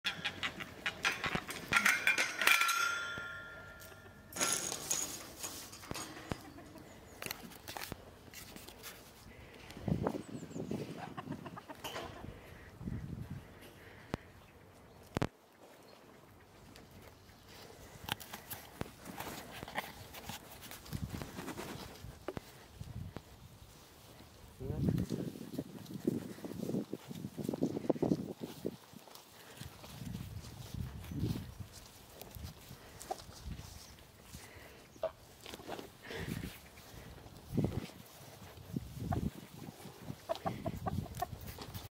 Chicken and Geese runnig to me
My Chicken and the Geese that used to live with them greeting me because they know I have food.